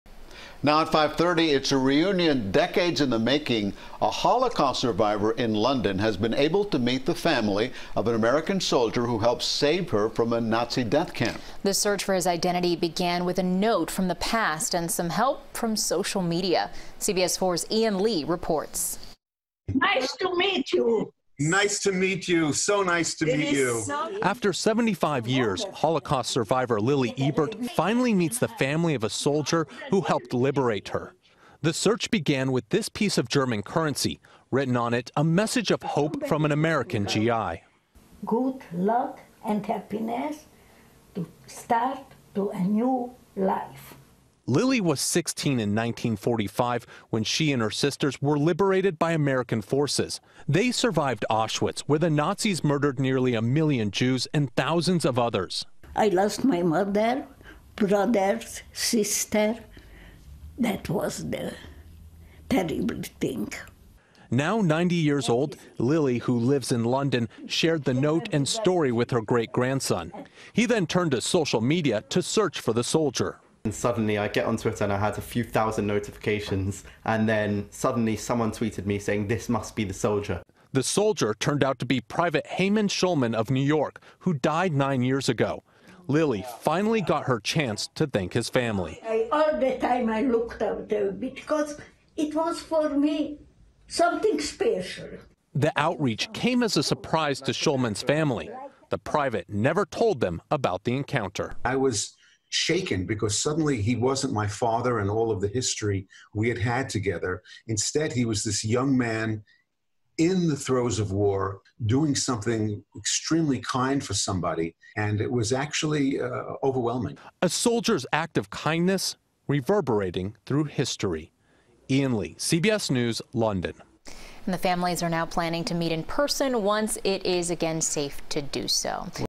Holocaust Survivor Meets Family Of Soldier Who Saved Her
CBS4's Ian Lee shares Lily Ebert's story.